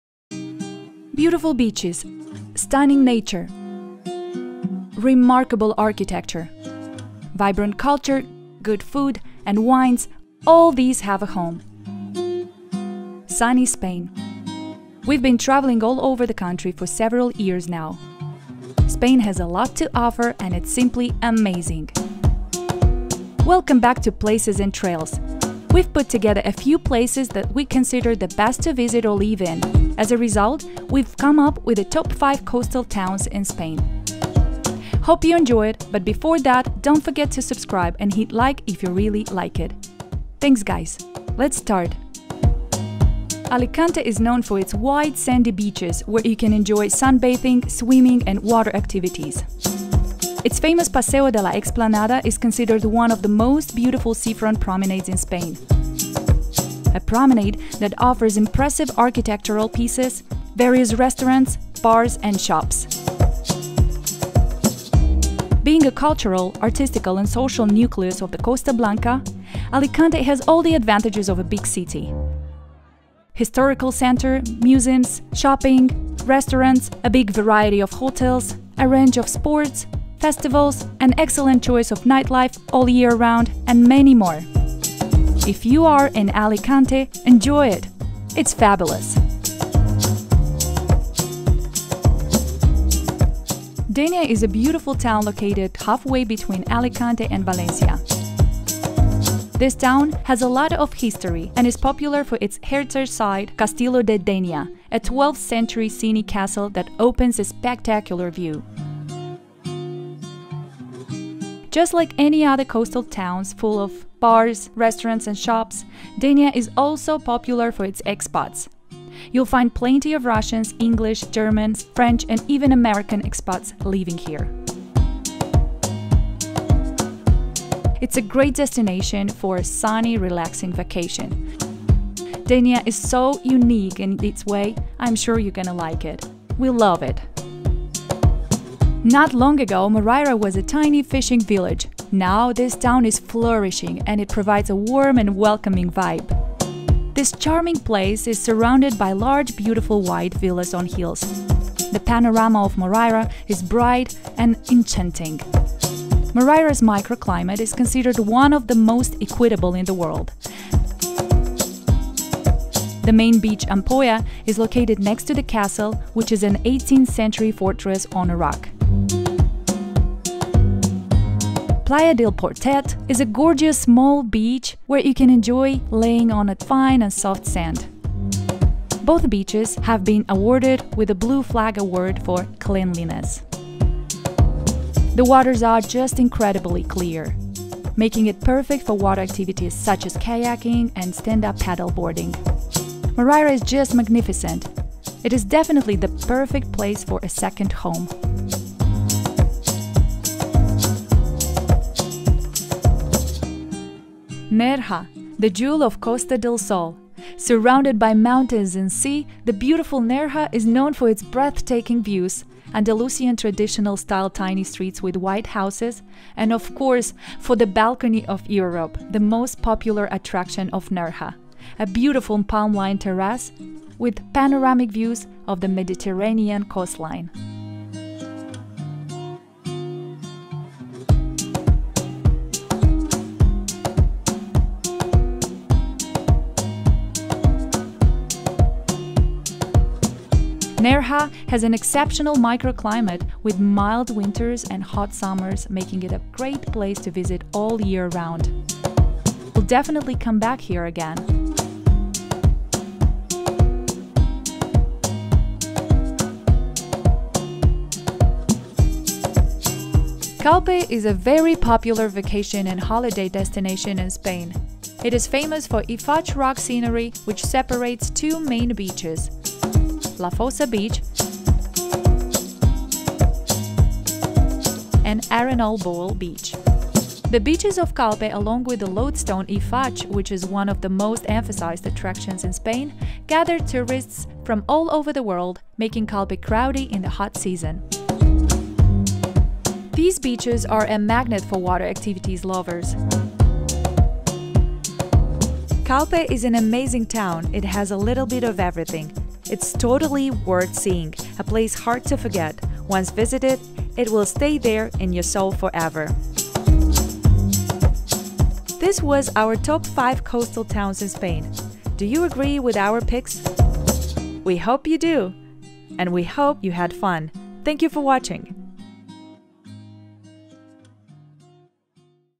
Top 5 Coastal Towns in Spain
Beautiful beaches, stunning nature, remarkable architecture, vibrant culture, good food and wines, all these have a home – Sunny Spain.
We’ve been traveling all over the country for several years now. Spain has a lot to offer and it is simply amazing! We’ve put together a few places that we consider the best to visit or live in. As a result, we’ve come up with top 5 coastal towns in Spain, which are Alicante, Denia, Moraira, Nerja and Calpe. Hope you enjoy it!

CHAPTERS:
00:00 Intro
00:43 Alicante
01:32 Denia
02:19 Moraira
03:23 Nerja
04:20 Calpe